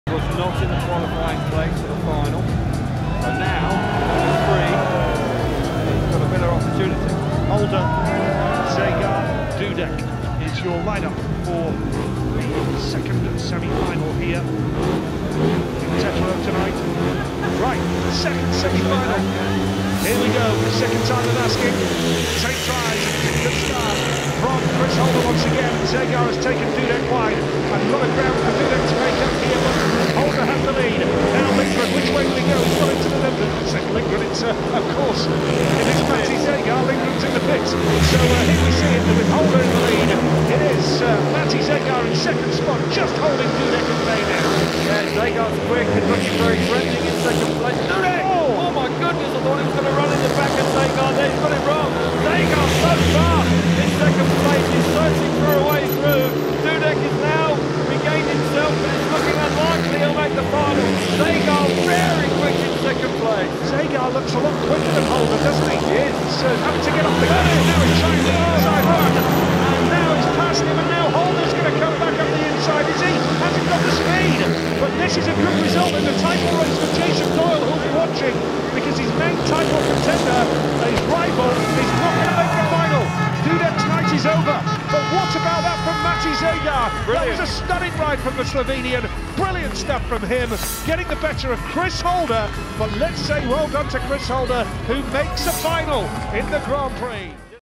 SGP Throwback: 2017 German SGP semi-final - Zagar v Holder
We rewind to this 2017 German SGP semi-final shoot-out between Matej Zagar and Chris Holder ...